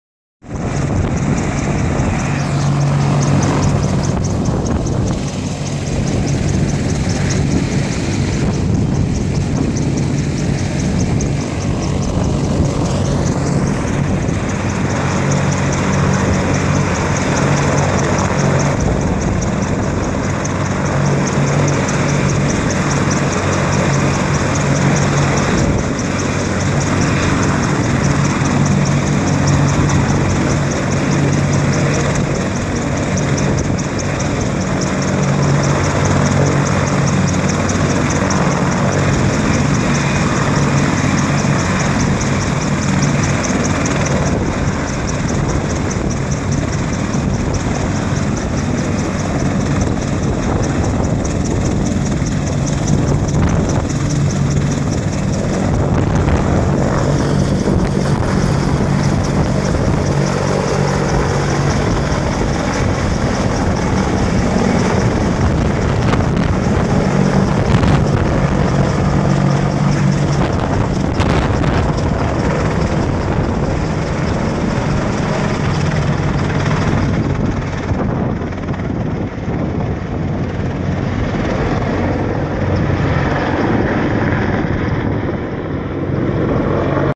Apache takes off from PNC Arena
The NHL Carolina Hurricanes hosted military appreciation day on February 28, 2016. This Apache helicopter (AH-64) was parked out near the arena and took off after the conclusion of the game.